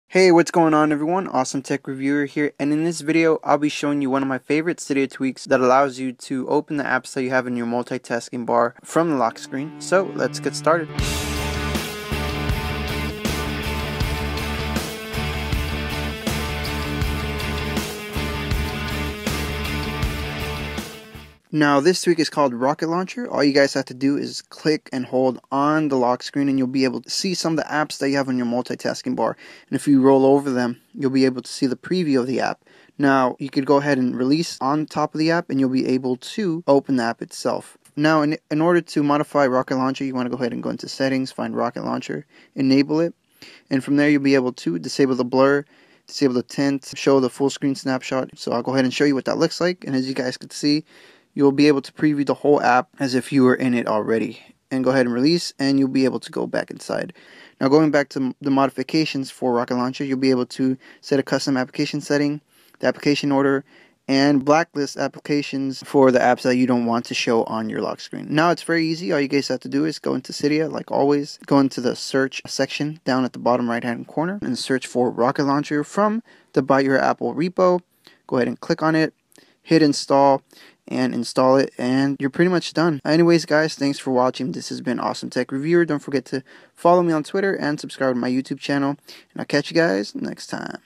iOS 7: App Launcher (Jailbreak Tweak) for iPhone, iPad, and iPod Touch
Clean status bar setting toggle cydia tweak for iPhone, iPad, and iPod Touch.

RocketLauncher- A neat unique way to view and open your apps already running on your iOS device!

Steps:

1.) Open Cydia.

2.) Click on search.

3.) Search for "Rocketlauncher" from the biteyourapple repo.

4.) Click install.

5.) Go into settings to configure

6. ) You're done!

Enjoy! :D